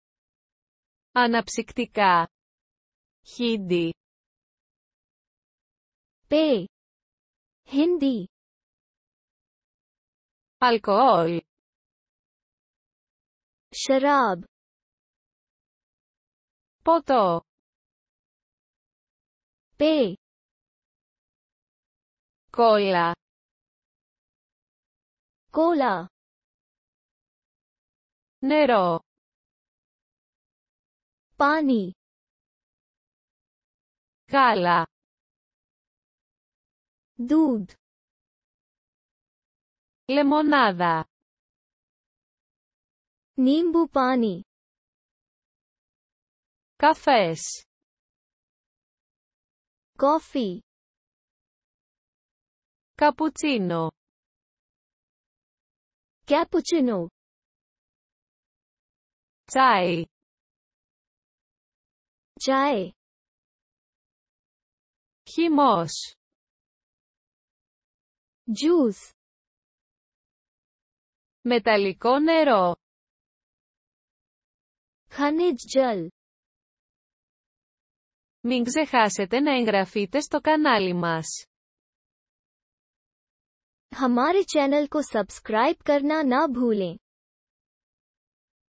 Αναψυκτικά (Χίντι) (el-hi)
Fone TV
el-hi-3
पेय (हिन्दी)
Αλκοόλ - शराब
Ποτό - पेय
Κόλα - कोला
Νερό - पानी
Γάλα - दूध
Λεμονάδα - नींबू पानी
Καφές - कॉफ़ी
Καπουτσίνο - कैपुचिनो
Τσάι - चाय
Χυμός - जूस
Μεταλλικό νερό - खनिज जल